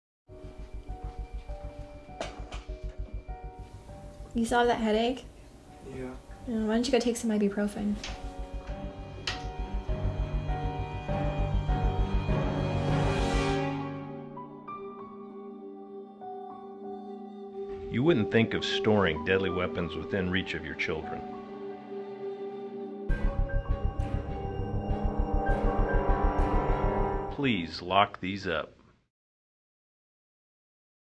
Medicine Cabinet
Parents: substance abuse is not just about the use of illicit drugs; it includes the abuse of prescription medications. Ensure all medications are secured and out of the hands of minors in your home.